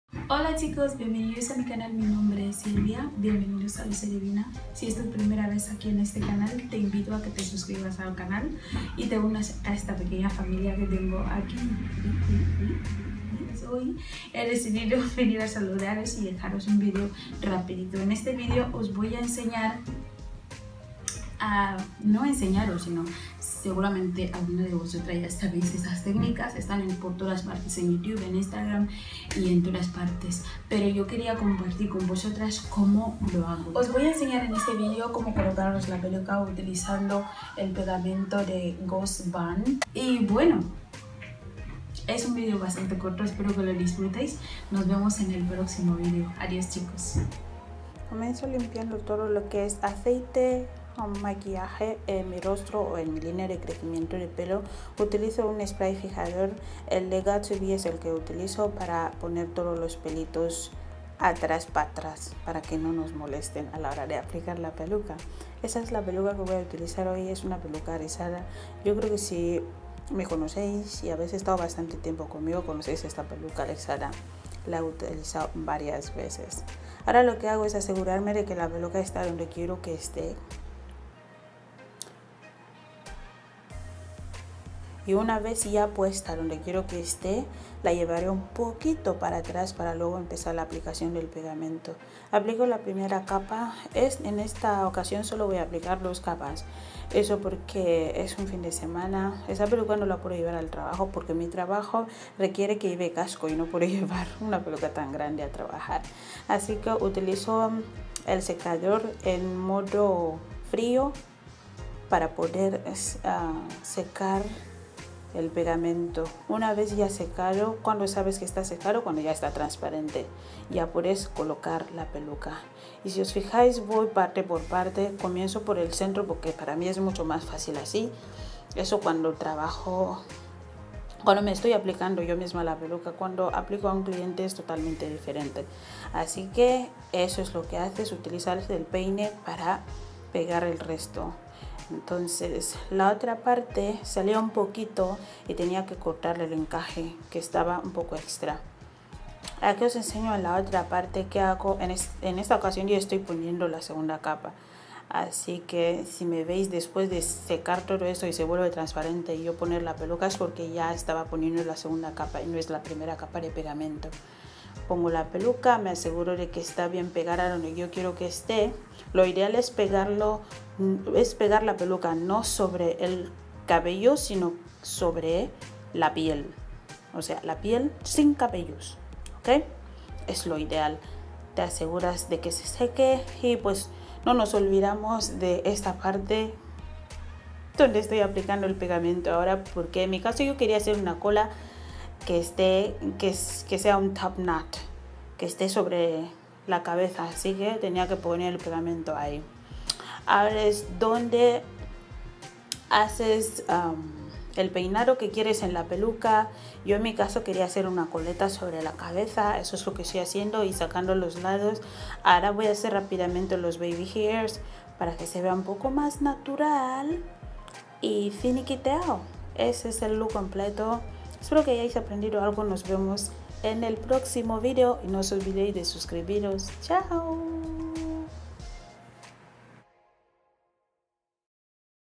Como me pongo la peluca usando el pegamento Ghost Bond// Luce Divina
hola chica/os hace ratoq ue hablamos ; pues aqui os dejo in video de como me ongo la peluca espero os sirva la informacion del video. XOXo